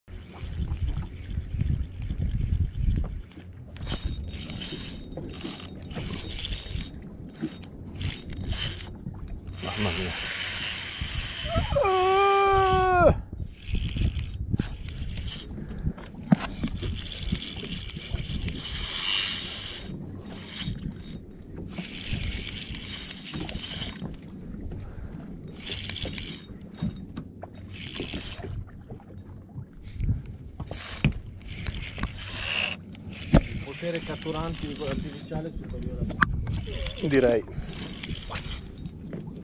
"ULTRA LIGHT" su mangianze.. Qualche secondo del suono più bello del mondo !
mangianze di piccoli tonni "rosso" favorite 10gr ,certate 2500R